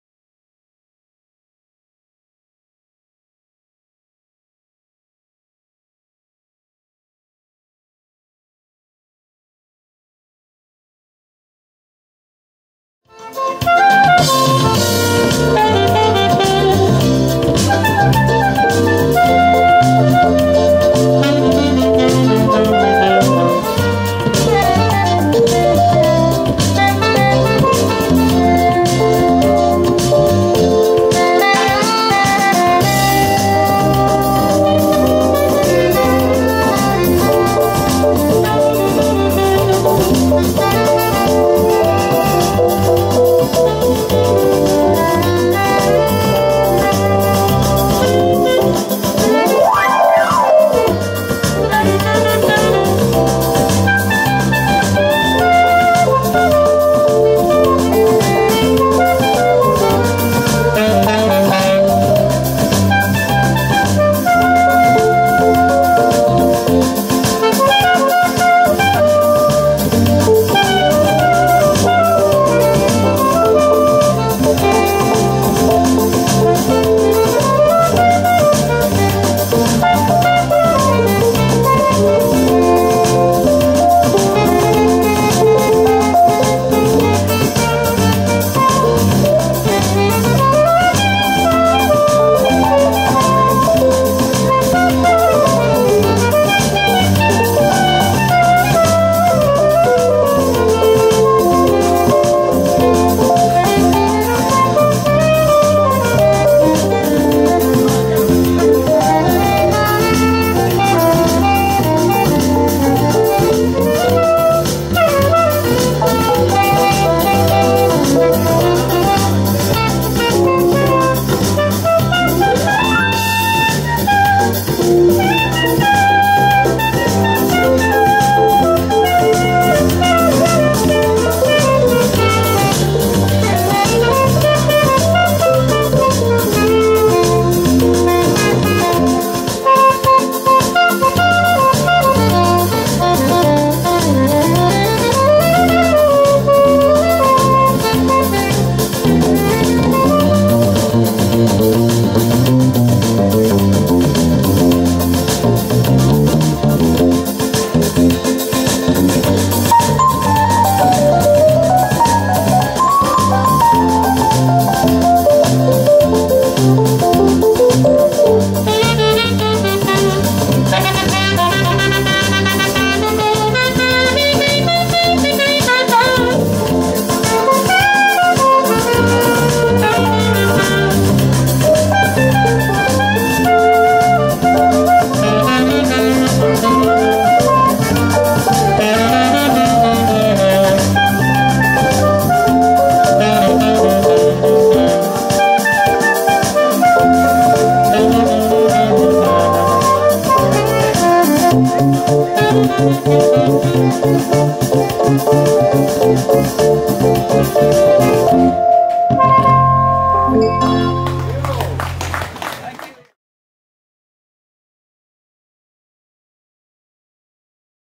Jazz Junction featuring Rhys Dsouza
Jazz by the Bay at Bay15, Odxel beach, Donna Paula, Goa.
Every Tuesday 7.30pm onwards.